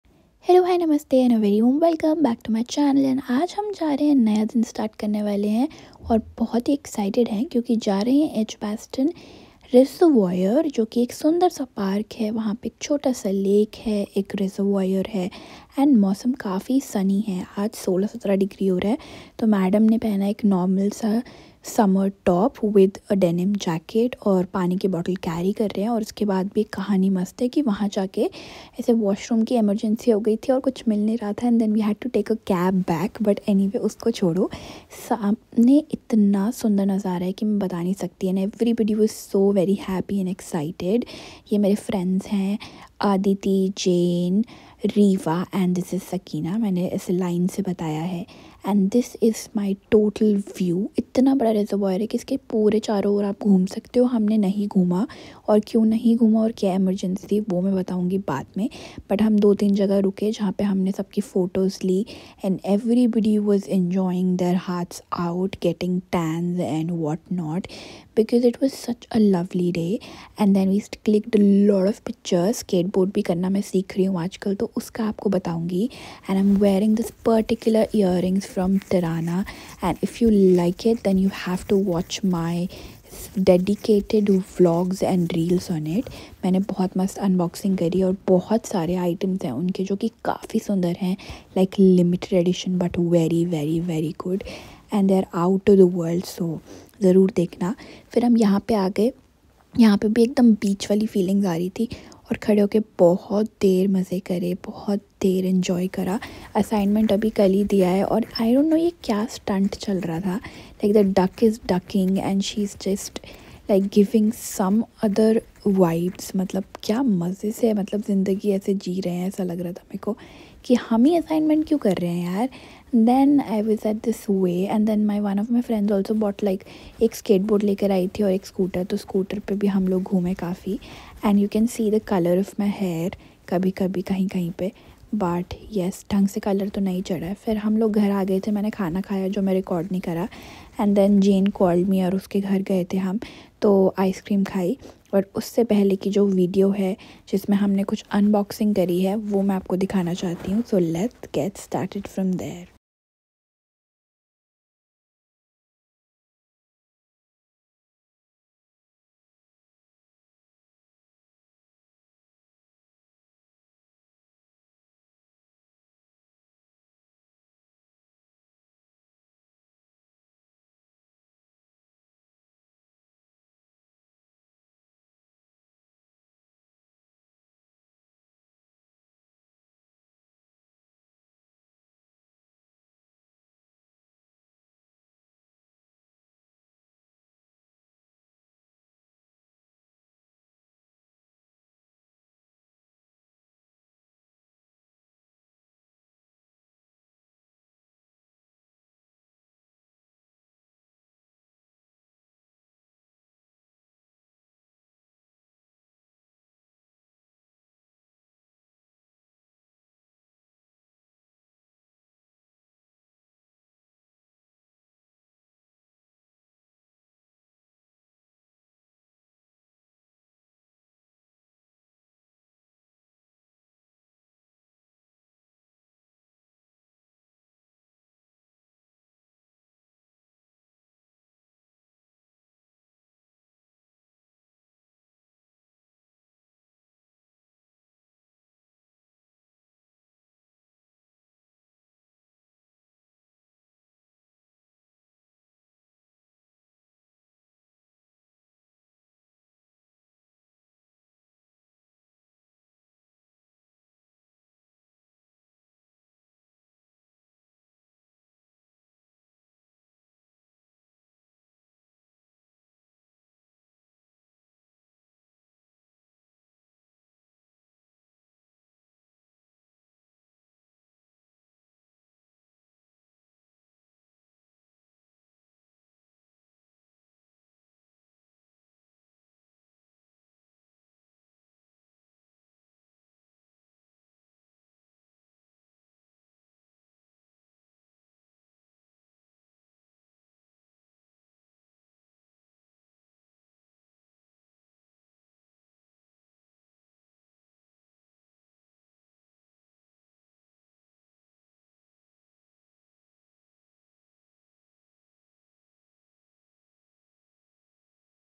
Indian Students Abroad *A day in my Uni Life* *international student* *UK life*
Hope to become a better human everyday:)
Xoxo
Samiksha :)